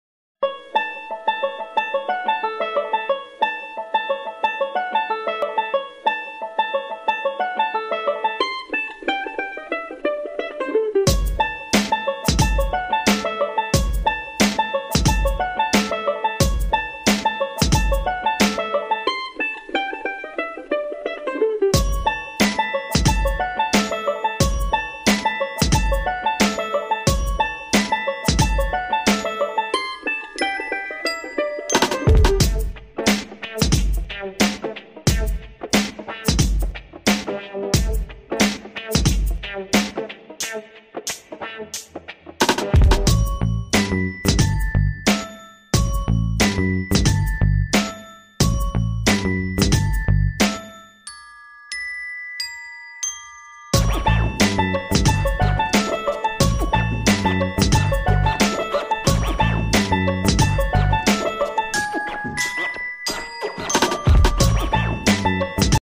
Laptop Returning Procedure
We made a quick video on how students will return their laptops on Monday.